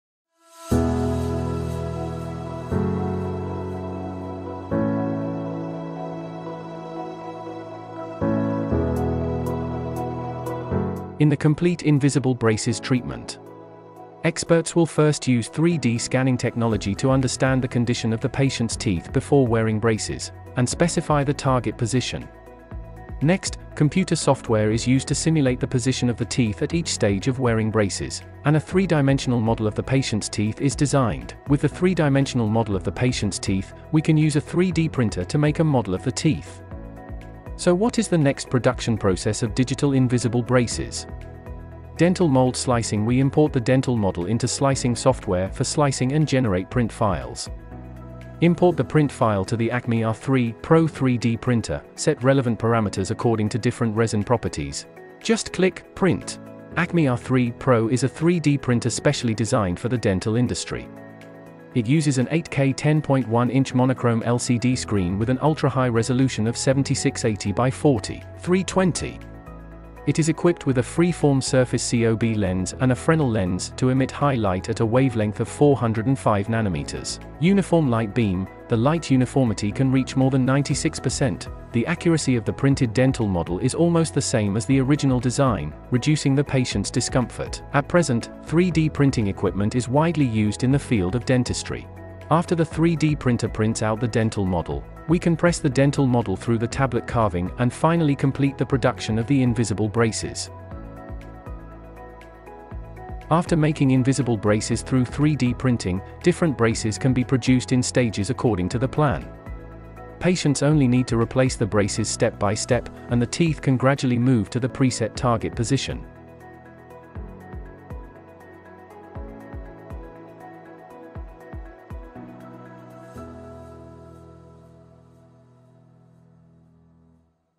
Acme R3 Pro 8K UItra-High Definition Dental 3D PRINT INVISIBLE BRACES Production Process
In the complete invisible braces treatment, experts will first use 3D scanning technology to understand the condition of the patient's teeth before wearing braces, and specify the target position. Next, computer software is used to simulate the position of the teeth at each stage of wearing braces, and a three-dimensional model of the patient's teeth is designed. With the three-dimensional model of the patient's teeth, we can use a 3D printer to make a model of the teeth.